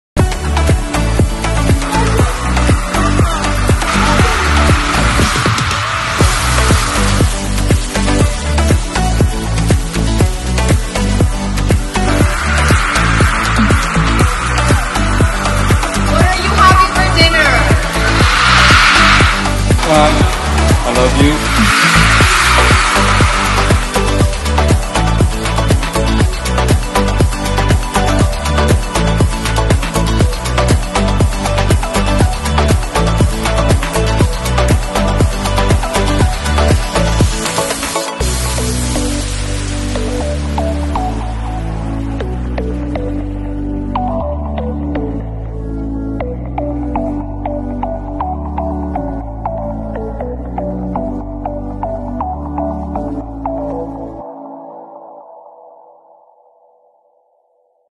ParkSeoJunForBENCH moments Part 2
psjphilippines Repost @benchtm
Still feels like yesterday‼️so here’s a little something to pull you back from moving on 😜 Hit ▶️ to relive your fave ParkSeoJunForBENCH moments! Got more pics/vids during the Fun Meet? Tag us @benchtm
P.S. We miss you, oppa @bn_sj2013! 🥰❣️